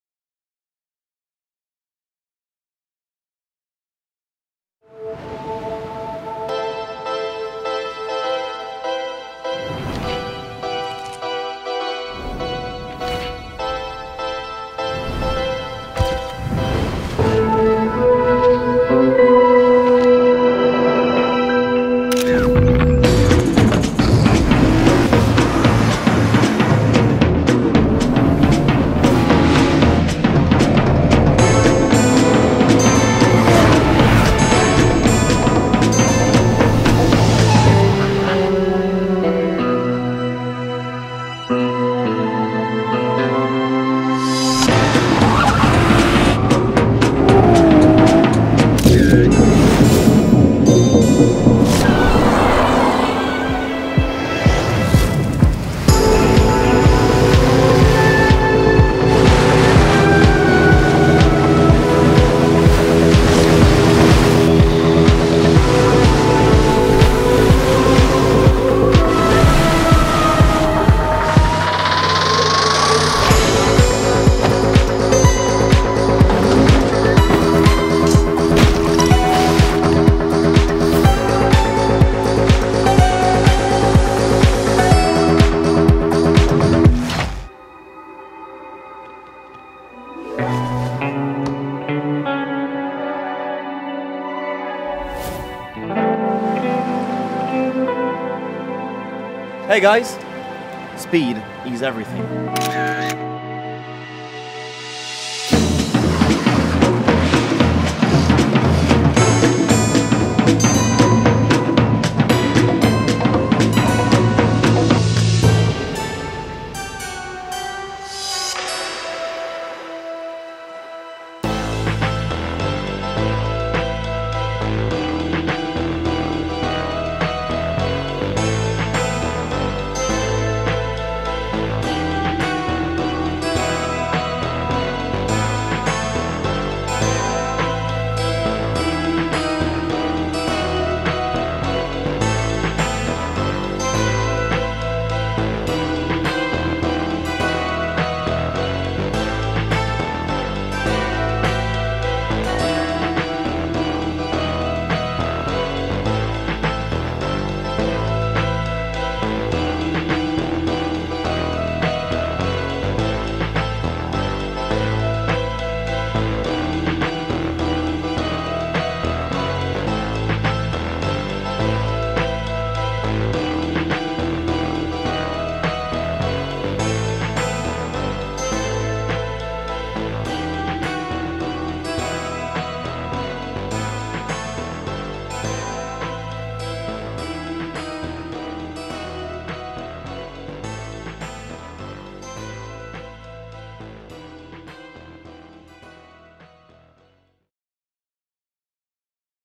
Riva: The Persuaders!
Italian actor Pierfancesco Favino and English football icon David Beckham race Ferrari cars and Riva yachts around the Côte d’Azur, with F1 star Charles Leclerc of Monaco joining in the fun.